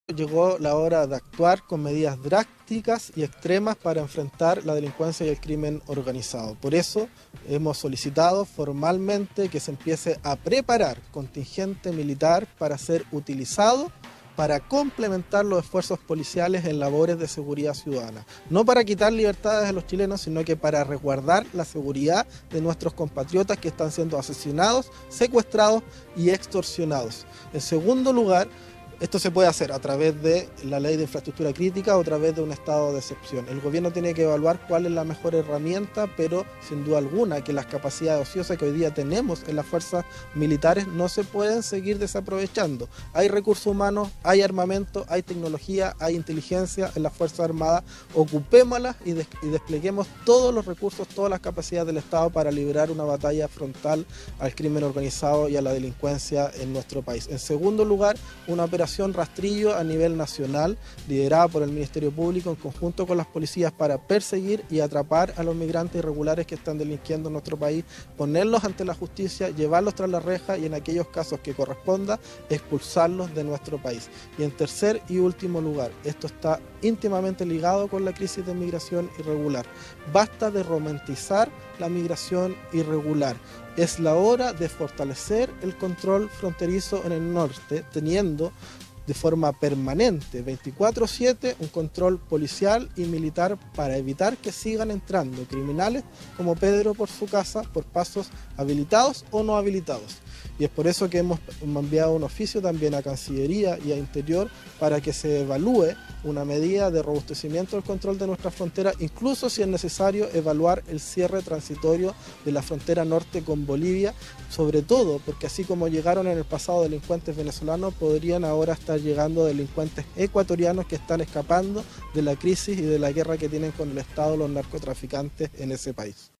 Raúl Soto solicita uso de contingente militar
Diputado PPD.